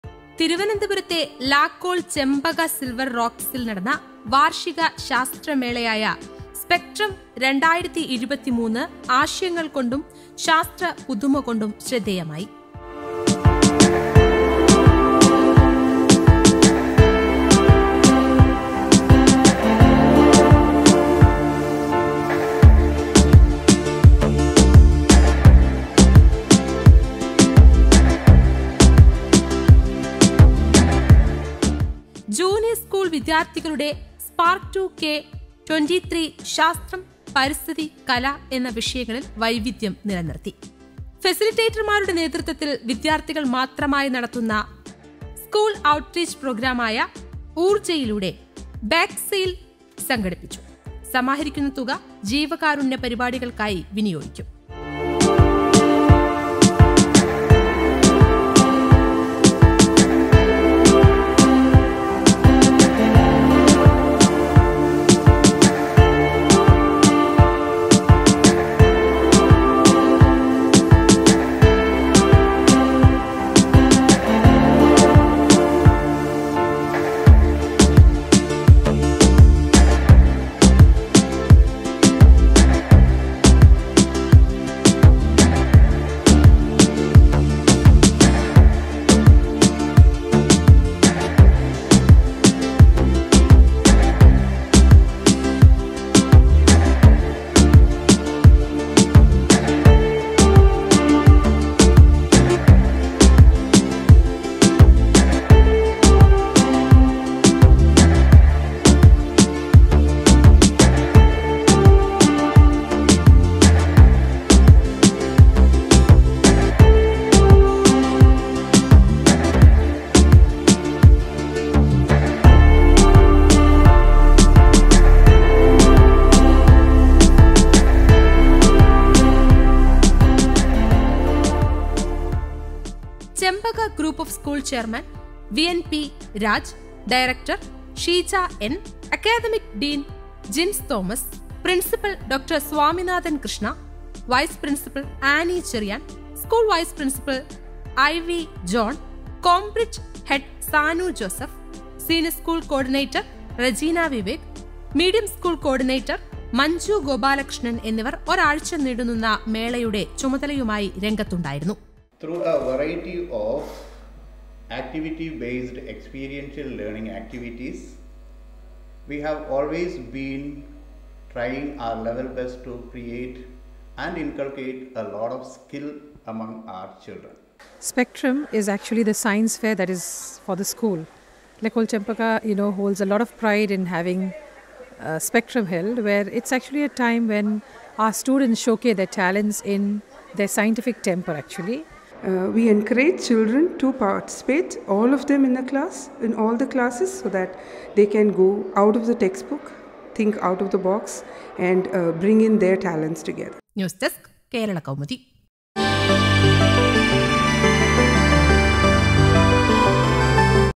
ആശയങ്ങൾ കൊണ്ട് തിളങ്ങി 'സ്‌പെക്ട്രം 2023' | Spectrum 2023 | L'école Chempaka International
തിരുവനന്തപുരത്തെ  ലാക്കോൾ  ചെമ്പക സിൽവർ റോക്‌സിൽ നടന്ന   വാർഷിക ശാസ്ത്ര മേളയായസ്പെക്ട്രം 2023  ആശയങ്ങൾ കൊണ്ടും  ശാസ്ത്ര പുതുമകൊണ്ടു ശ്രേദ്ധേയമായി.
  ജൂനിയർ സ്‌കൂൾ വിദ്യാർത്ഥികളുടെ സ്പാര്ക് ടു കെ 23  'ശാസ്ത്രം, പരിസ്ഥിതി, കല' എന്ന വിഷയങ്ങളിൽ വൈവിധ്യം  നിലനിർത്തി..

ഫെസിലിറ്റേറ്റർമാരുടെ നേതൃത്വത്തിൽ വിദ്യാർത്ഥികൾ മാത്രമായി നടത്തുന്ന സ്‌കൂൾ ഔട്ട്‌റീച്ച് പ്രോഗ്രാമായ 'ഊർജ' യിലൂടെ  ബേക്ക്-സെയിൽ സംഘടിപ്പിച്ചു. സമാഹരിക്കുന്ന തുക ജീവകാരുണ്യ പരിപാടികൾക്കായി വിനിയോഗിക്കും.